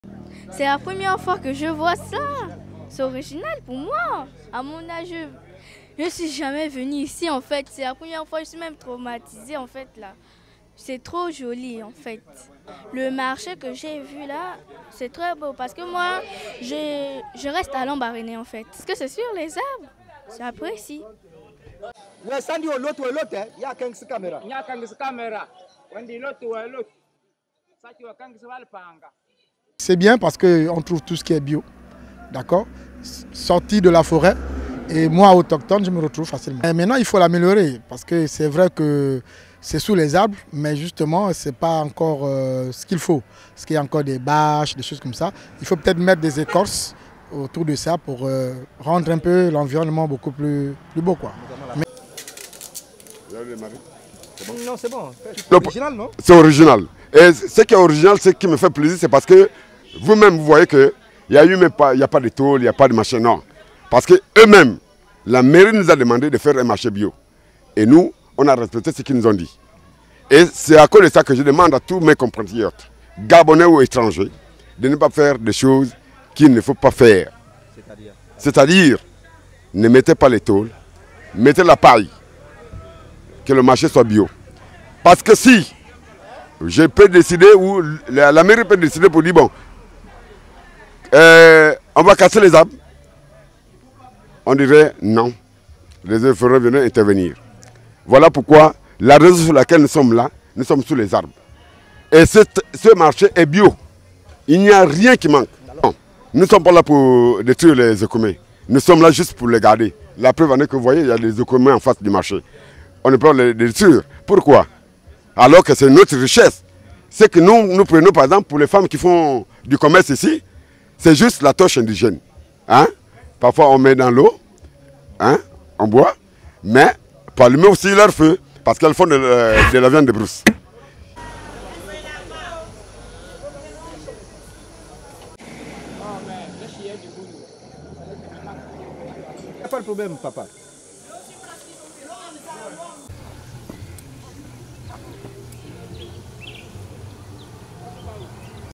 Gabon: sous la canopée de Cap Estérias, une curiosité naturelle qui n’a pas volé son nom
A une vingtaine de kilomètres au nord de Libreville, Cap Estérias est un petit village de bord de mer à la végétation luxuriante, un espace idéal pour les randonnées et les découvertes gastronomiques. Au menu, un bol d’oxygène frais et des produits naturels.